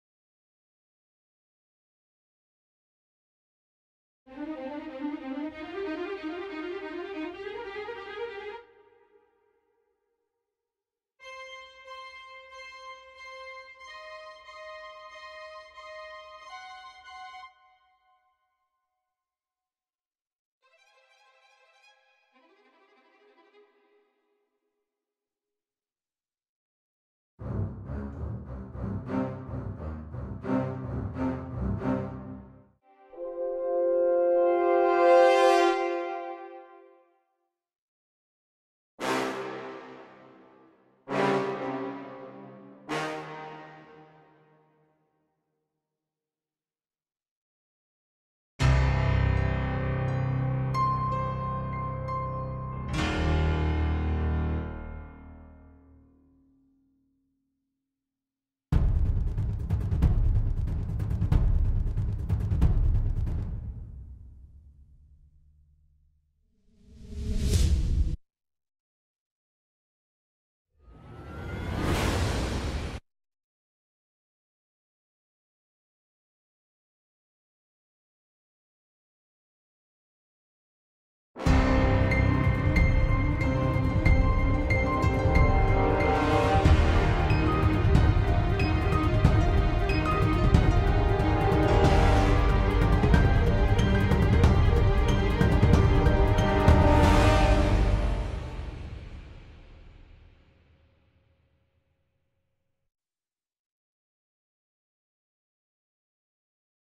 VSTi In Action - Komplete 12 Hollywood Brass 8Dio Agitato Legato
Very short demo and track:

REAPER 6
NI EMOTIVE STRINGS
NI SYMPHONY ESSENTIALS BASSES Stacc
HOLLYWOOD BRASS GOLD
NI THE GIANT piano
NI ACTION STRINGS
NI RISE AND HIT

I hope you enjoy! I'd love to hear and tips about how I could improve these videos.
Thanks!